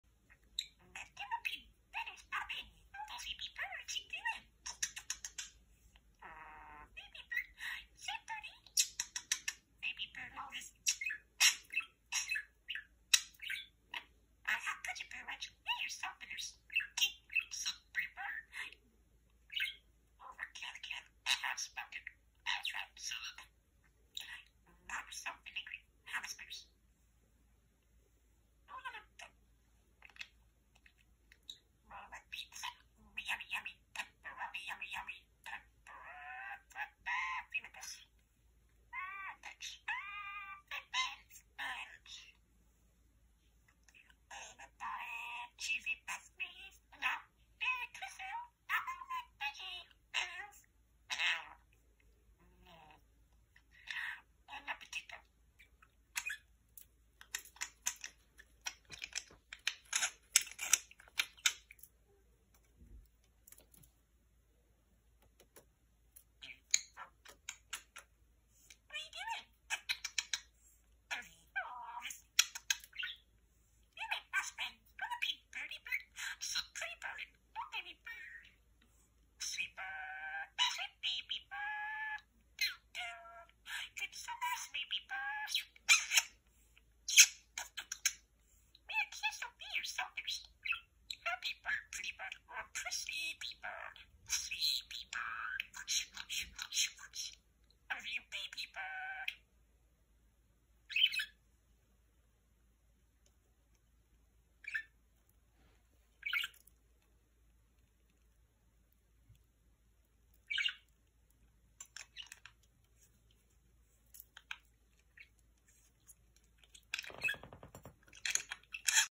Just a little chat to the lamp
Talking parakeet shares some of his favorite phrases.